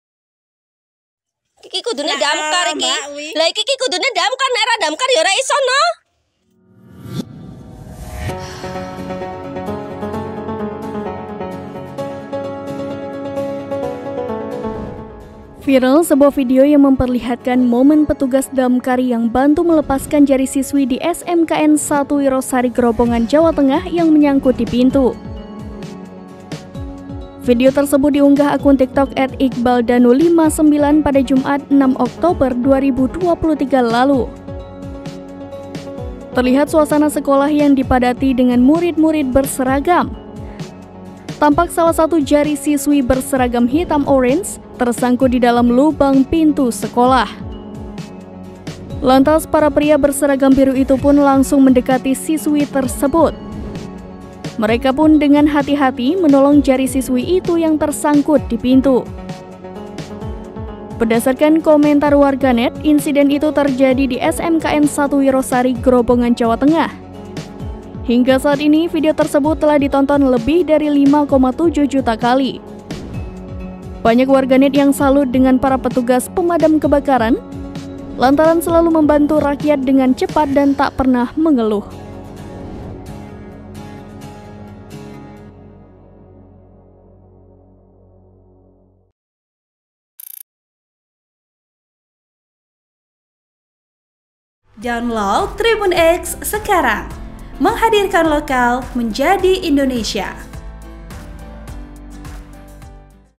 Viral Petugas Damkar Bantu Lepaskan Jari Siswi yang Tersangkut di Dalam Pintu Sekolah
SERAMBINEWS.COM - Viral sebuah video memperlihatkan momen petugas damkar yang bantu melepaskan jari siswi di SMKN 1 Wirosari, Grobogan, Jawa Tengah yang menyangkut di pintu.

Video tersebut diunggah oleh akun TikTok @iqbaldanu59 pada Jumat (6/10/2023) lalu.

Terlihat suasana sekolah yang dipadati dengan murid-murid berseragam. Tampak salah satu jari siswi berseragam hitam orange tersangkut di dalam lubang pintu sekolah.

Lantas para pria berseragam biru itu langsung mendekati siswi tersebut. Mereka pun dengan hati-hati menolong jari siswi itu yang tersangkut di pintu.

Berdasarkan komentar warganet, insiden itu terjadi di SMKN 1 Wirosari, Grobogan, Jawa Tengah. Hingga kini video tersebut telah ditonton lebih dari 5,7 juta kali.

Banyak warganet yang salut dengan para petugas pemadam kebakaran, lantaran selalu membantu rakyat dengan cepat dan tak pernah mengeluh.(*)